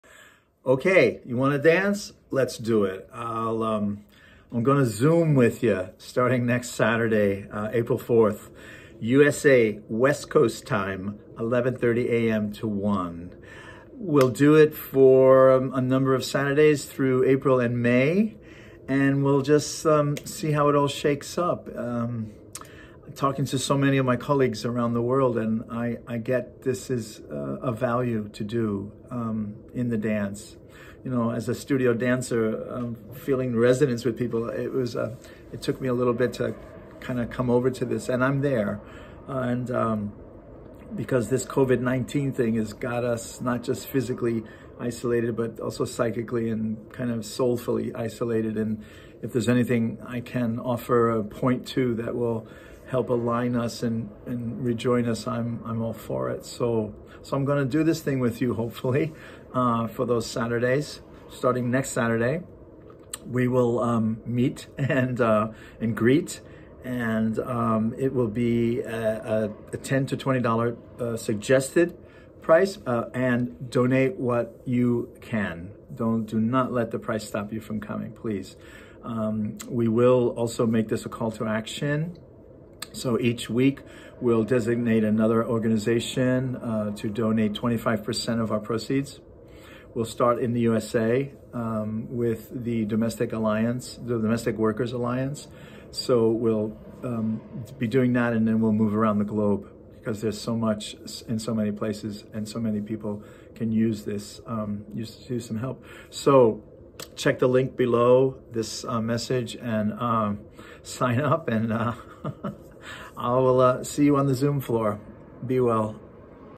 Online Classes with Vincent (Arjuna) in April and May 2020
You want to dance? Let’s do it!

Traditionally an in-person, in-studio dancer and teacher, Vincent’s going to bring his movement inquiries to Zoom. Talking to so many of his colleagues around the world, it’s become clear to him that it’s of value to bring dance to you with this new platform.

COVID-19’s got us not only physically isolated but also soulfully and psychically isolated, and he’s hoping that this offering can help to align us and join us within the conscious dance community.